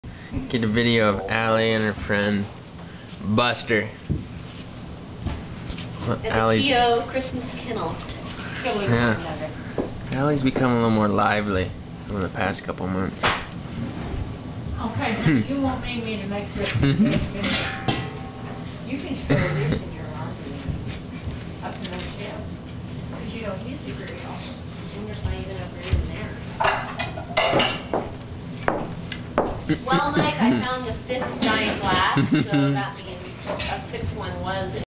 Allie at Home
Short video of Allie who was our adoption # 279.